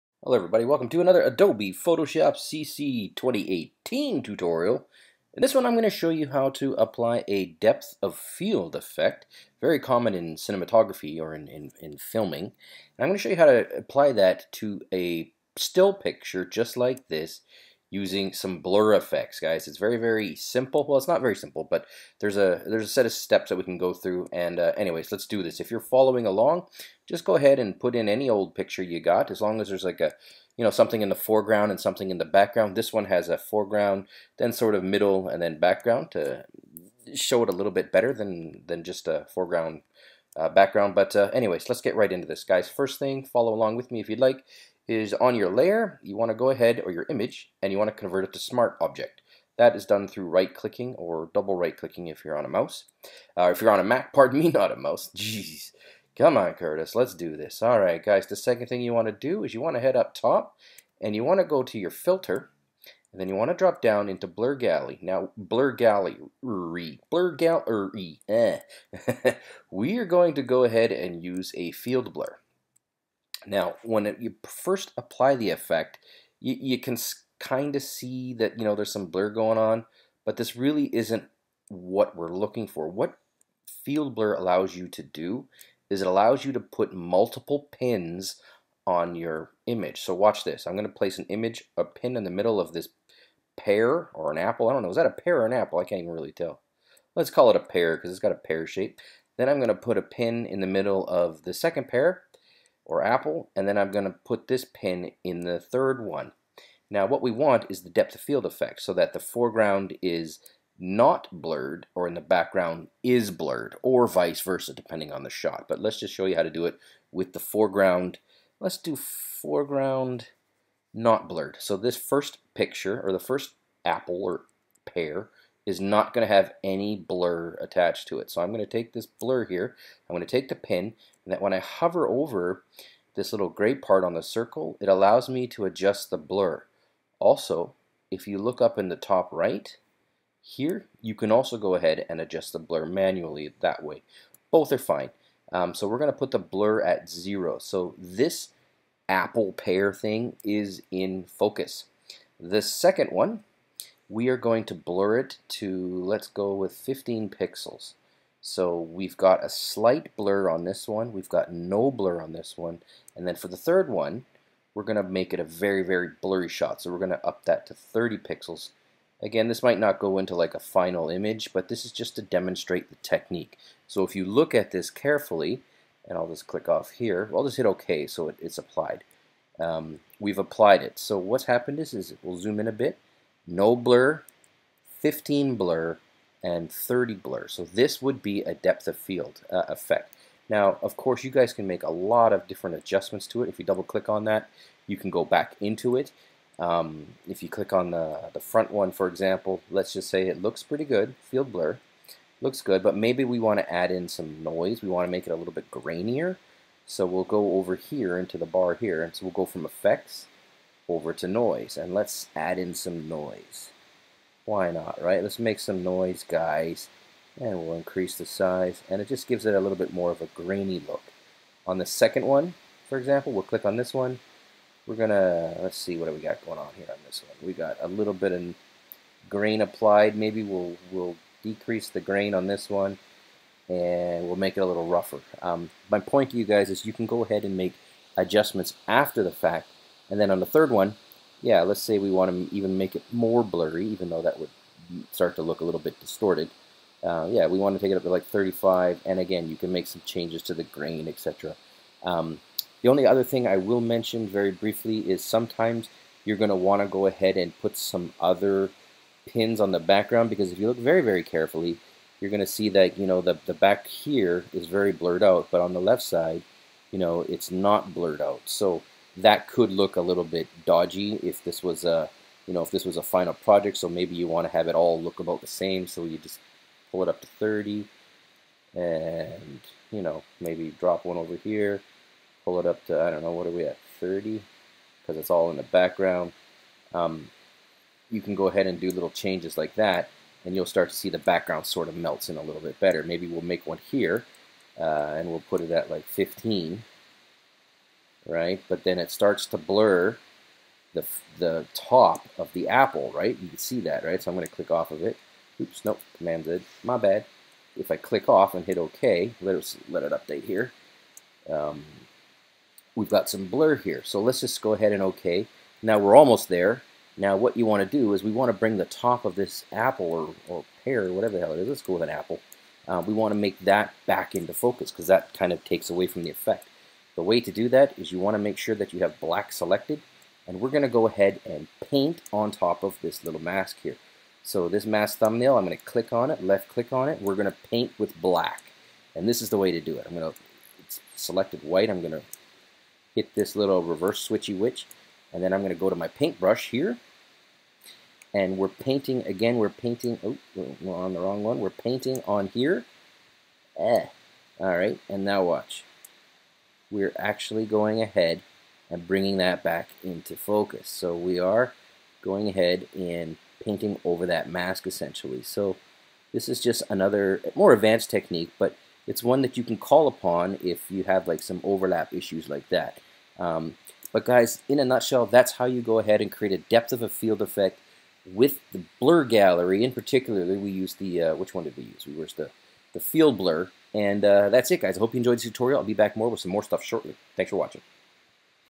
Adobe Photoshop CC - How to create depth of field
Here is a quick tutorial showing you how to create a depth of field effect inside Adobe photoshop CC 2018.  I used the field blur effect along with multiple pins and varying degrees of blur.